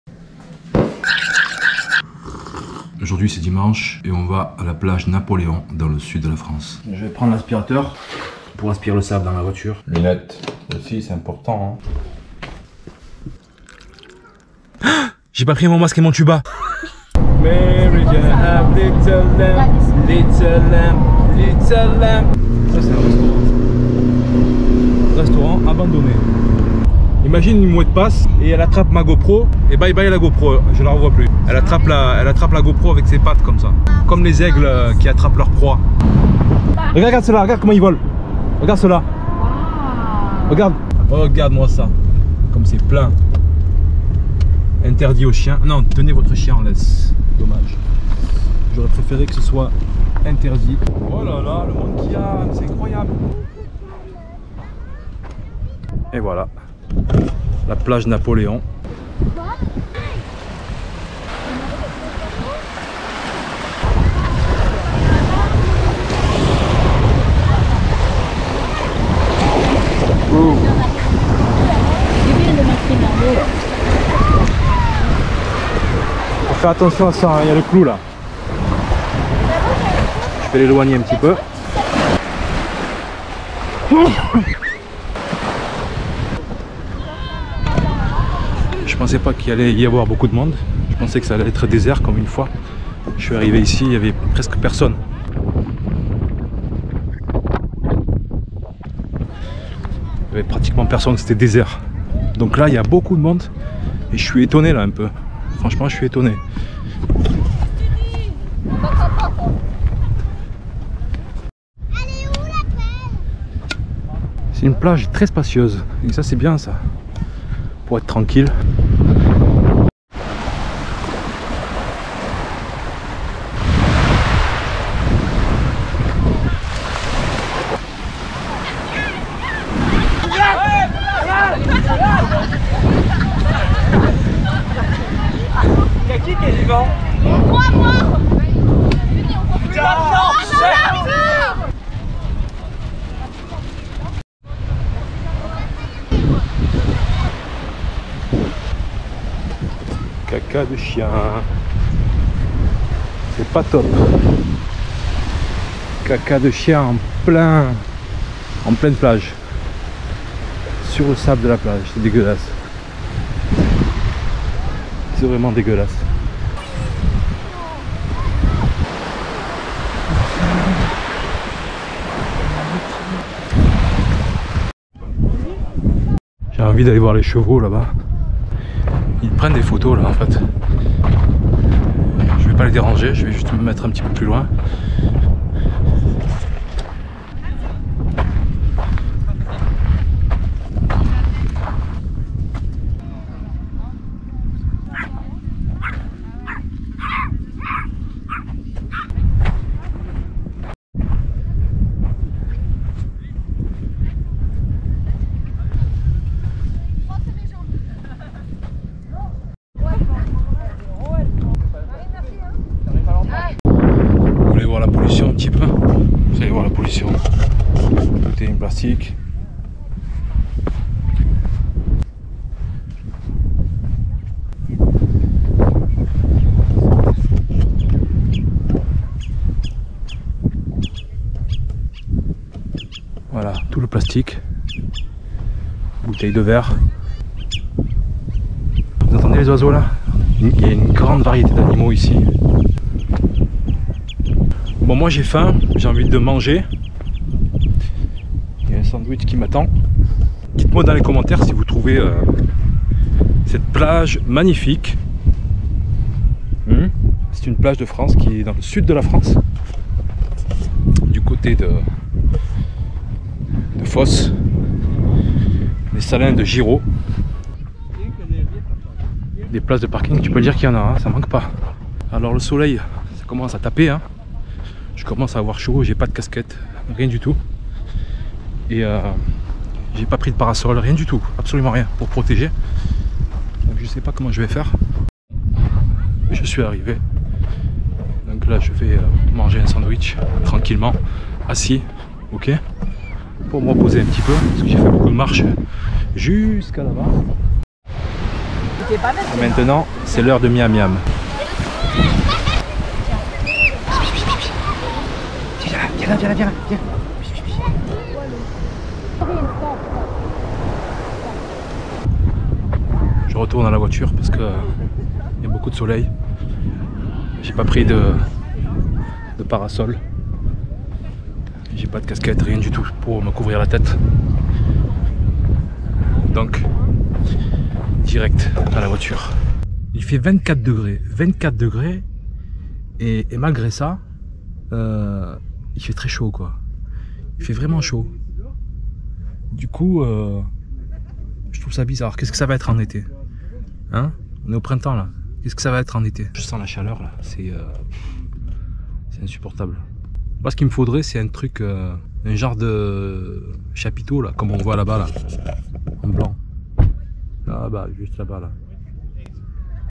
DES CROTTES DE CHIEN DANS LES PLAGES DE FRANCE / vlog 24
En période estivale les plages de France sont très fréquentées. Des familles vont y passer toute la journée, laissant leurs enfants construire des châteaux de sable. Les plages de France sont très négligées quant à leur hygiène. Surtout les plages autorisées aux chiens... Les déjections canines posent de sérieux problèmes pour la santé des enfants. Il faut savoir que "ces excréments véhiculent des parasites qui représentent un risque d’infection grave, en particulier pour les enfants", (DOCTEUR ARNAULT PFERSDORFF).

C'est du sérieux. La pollution fécale, en France, est une réalité. On ne peut pas faire comme si ce problème n'existait pas. Le chien est un réservoir d’agents pathogènes divers dont les enfants sont les premières victimes.

FAIT ÇA : Like le vlog si tu l'as aimé. Chaque fois que je publierai une nouvelle vidéo, tu seras notifié.